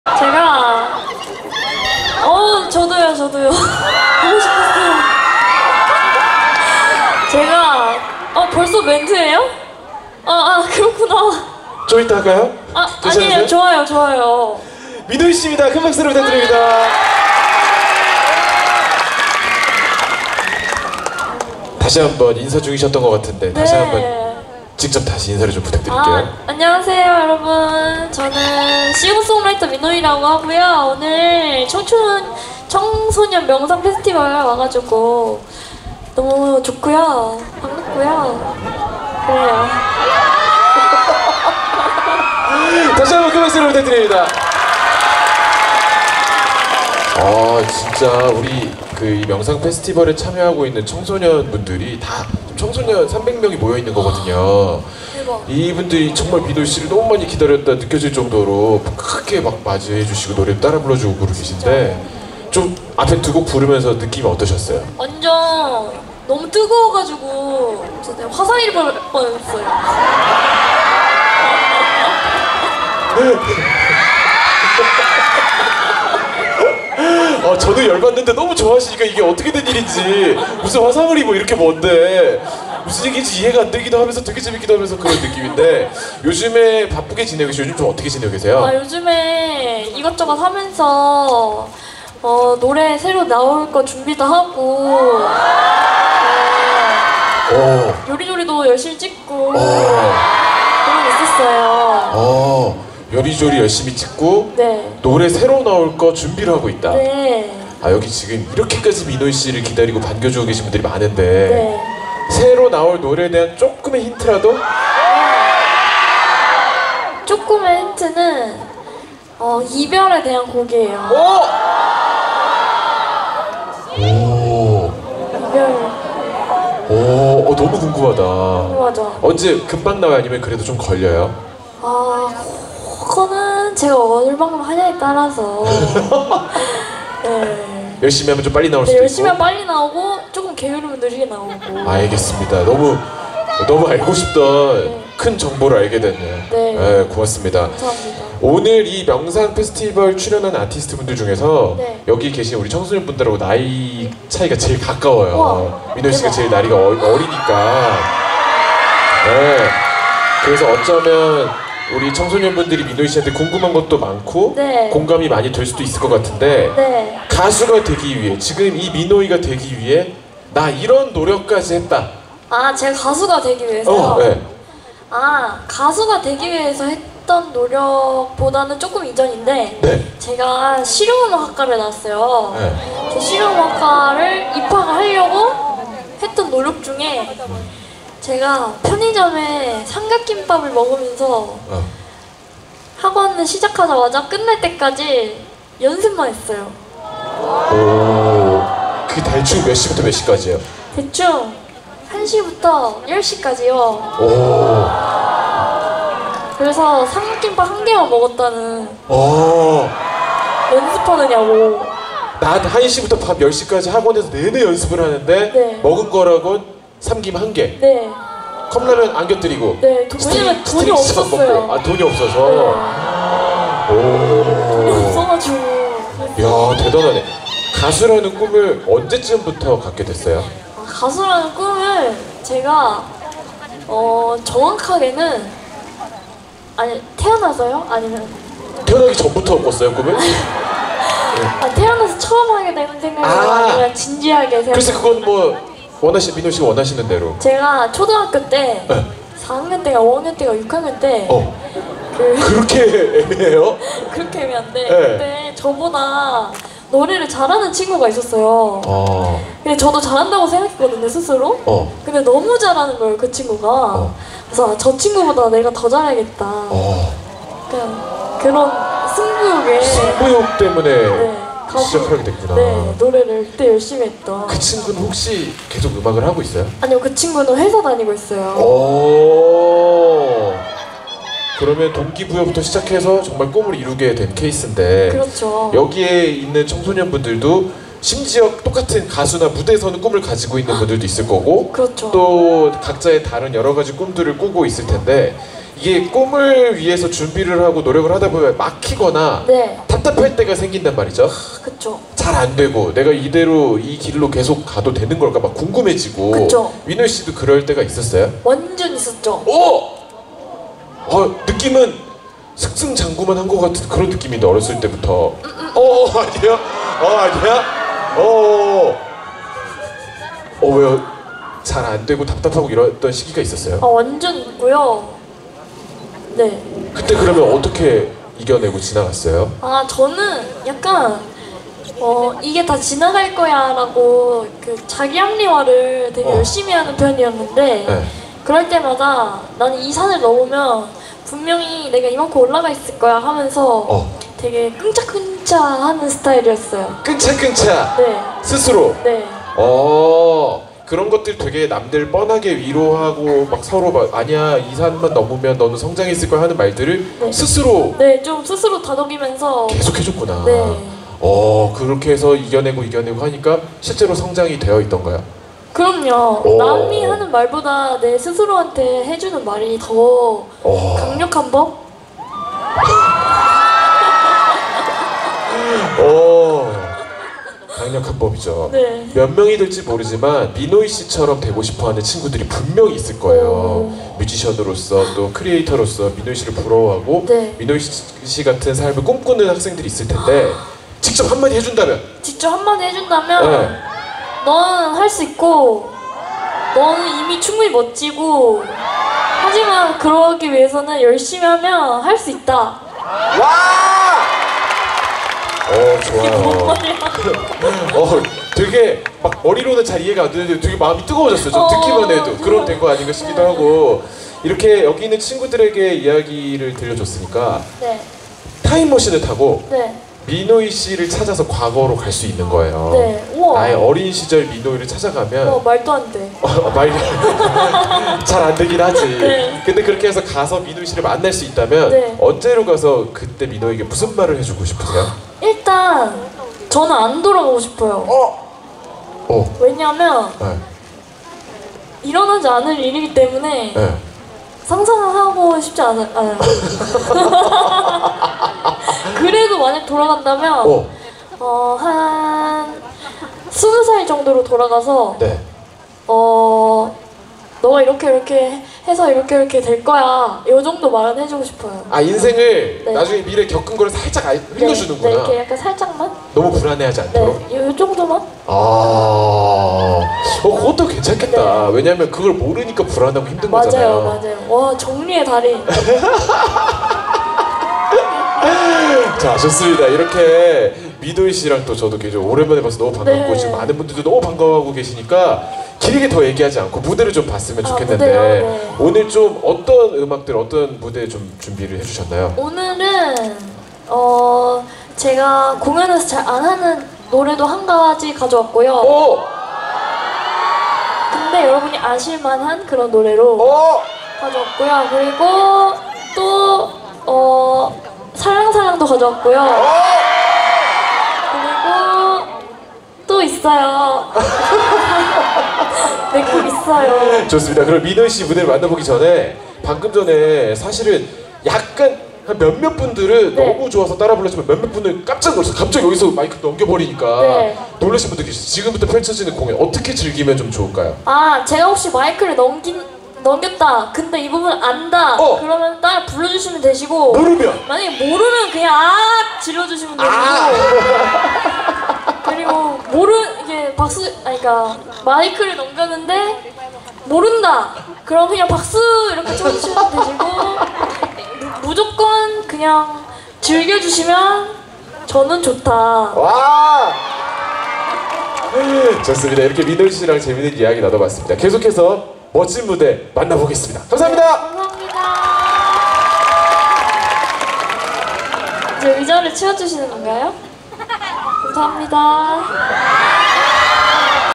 [220803][4K] 미노이 멘트 (with MC 고영배) @MEME 콘서트
220803 미노이(meenoi) 멘트 4K 직캠

fancam by 유일한하날